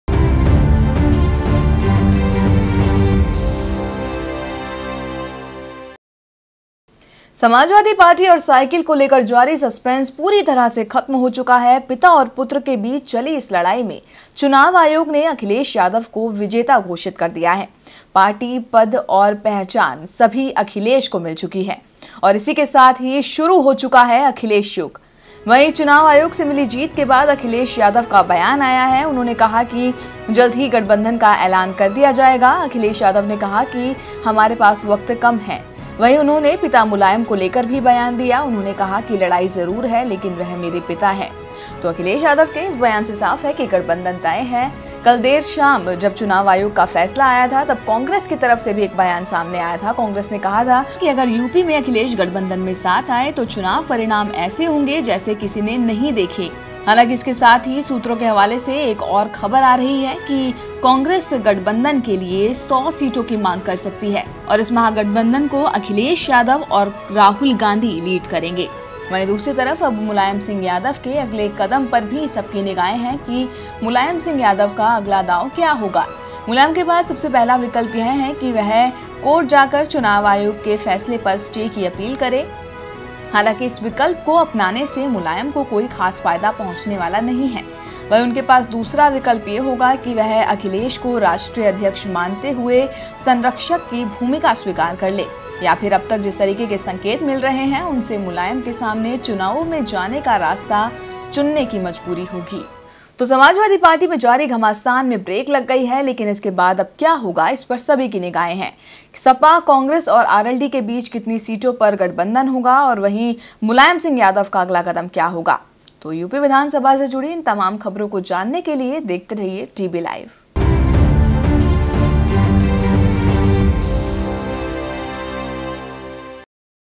DB LIVE | 17 JAN 2017 | UP elections: Congress-Akhilesh Yadav tie-up is next
समाजवादी पार्टी और साइकिल को लेकर जारी सस्पेंस पूरी तरह से खत्म हो चुका है। पिता और पुत्र के बीच चली इस लड़ाई में चुनाव आयोग ने अखिलेश यादव को विजेता घोषित कर दिया है। पार्टी, पद और पहचान सभी अखिलेश को मिल चुकी है और इसी के साथ ही शुरू हो चुका है अखिलेश युग। वहीं चुनाव आयोग से मिली जीत के बाद अखिलेश यादव का बयान आया है उन्होंने कहा है कि जल्द ही गठबंधन का ऐलान कर दिया जाएगा..अखिलेश यादव ने कहा कि हमारे पास वक्त कम है। वहीं उन्होंने पिता मुलायम को लेकर भी बयान दिया। उन्होंने कहा कि लड़ाई ज़रूर है, लेकिन वह मेरे पिता हैं। तो अखिलेश के बयान से ये साफ है कि गठबंधन तय है। कल देर शाम चुनाव आयोग का फैसला आते ही कांग्रेस की तरफ से भी एक बयान आया। कांग्रेस ने कहा कि यूपी में अखिलेश गठबंधन में साथ आए तो चुनाव परिणाम ऐसे होंगे जैसे किसी ने नहीं देखे होंगे। हालांकि इसके साथ ही सूत्रों के हवाले से एक और खबर आ गई कि कांग्रेस गठबंधन के लिए 100 सीटों की मांग कर सकती है...सूत्रों के मुताबिक महागठबंधन को अखिलेश यादव लीड करेंगे...वहीं दूसरी तरफ अब मुलायम सिंह यादव के अगले कदम पर भी सबकी निगाहे हैं...कि मुलायम सिंह यादव का अगला दांव क्या होगा... मुलायम के पास सबसे पहला विकल्प यह है कि वह कोर्ट जाकर चुनाव आयोग के फैसले पर स्टे की अपील करें। हालांकि इस विकल्प को अपनाने से मुलायम को कोई खास फायदा होने वाला नहीं है...वहीं उनके पास दूसरा विकल्प ये होगा कि वह अखिलेश को राष्ट्रीय अध्यक्ष मानते हुए संरक्षक की भूमिका स्वीकार कर लें...या फिर अब तक जिस तरीके के संकेत मिल रहे हैं, उनसे मुलायम के सामने चुनावों में जाने का रास्ता चुनने की मजबूरी होगी। तो समाजवादी पार्टी में जारी घमासान में ब्रेक लग गई है। लेकिन इसके बाद क्या होगा। इस पर सबकी निगाहें इस पर है..सपा, कांग्रेस और आरएलडी के बीच कितनी सीटों पर गठबंधन होगा और वहीं मुलायम सिंह यादव अब क्या करेंगे।